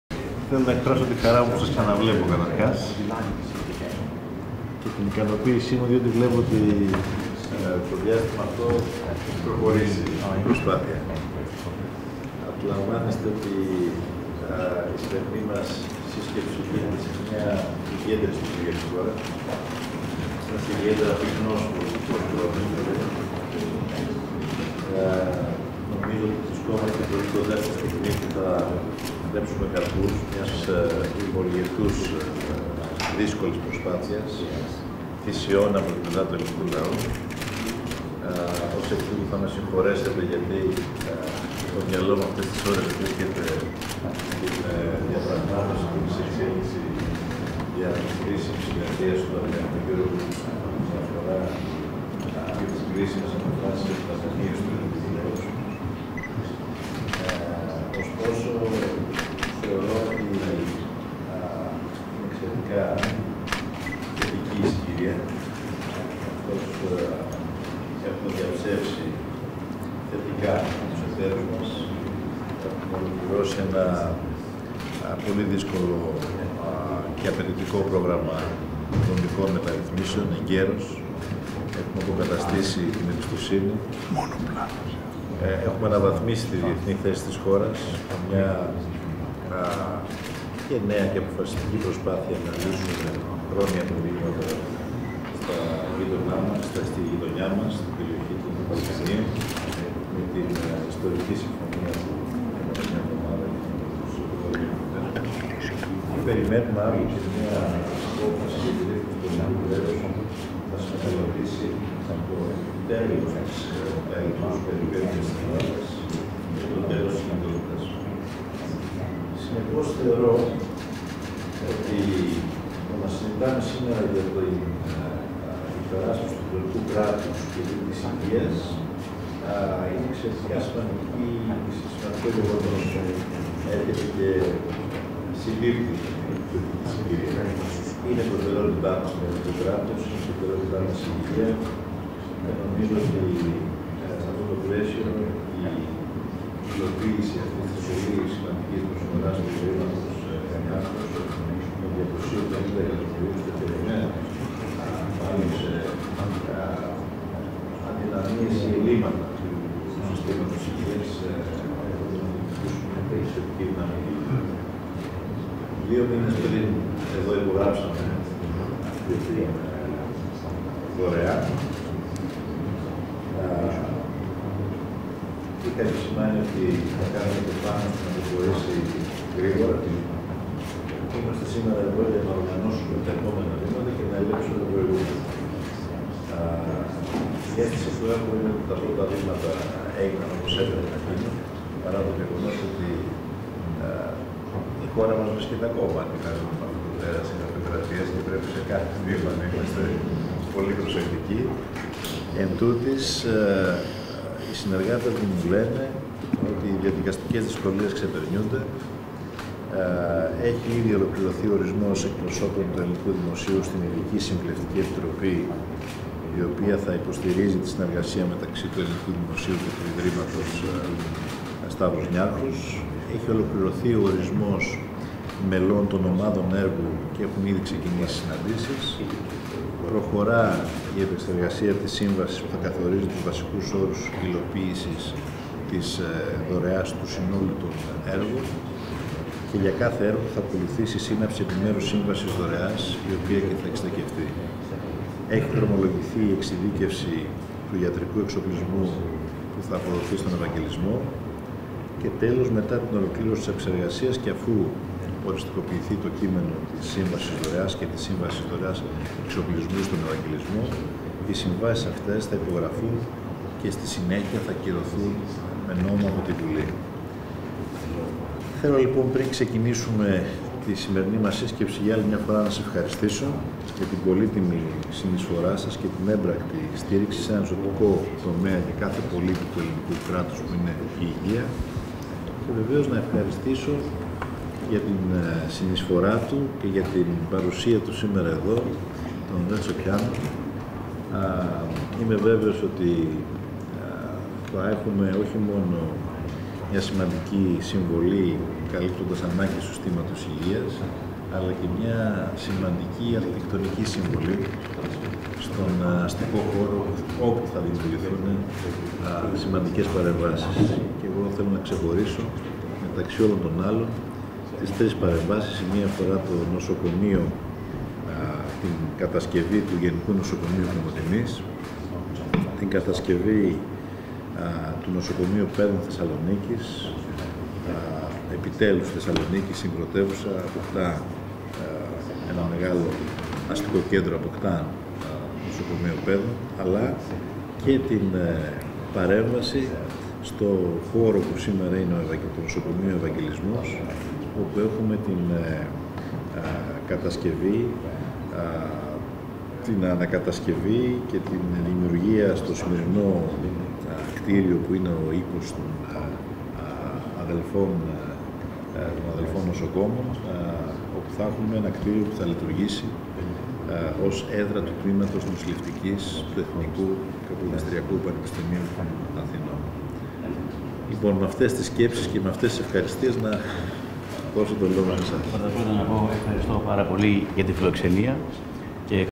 Συνάντηση με τον Πρόεδρο του Ιδρύματος Σταύρος Νιάρχος
Συνάντηση του Πρωθυπουργού Αλέξη Τσίπρα με τον Πρόεδρο του Ιδρύματος Σταύρος Νιάρχος, Ανδρέα Δρακόπουλο προκειμένου να ενημερωθεί για τον σχεδιασμό των έργων στον τομέα της Υγείας που έχει αναλάβει το Ίδρυμα με την υπογραφή σχετικού Μνημονίου Συνεργασίας με την Ελληνική Δημοκρατία τον Μάρτιο του 2018.
Στη συνάντηση συμμετέχουν ο αρχιτέκτονας που έχει αναλάβει τον σχεδιασμό των έργων υποδομής, Renzo Piano, καθώς και ο Πρόεδρος του Πανεπιστημίου John Hopkins, Ronald Daniels.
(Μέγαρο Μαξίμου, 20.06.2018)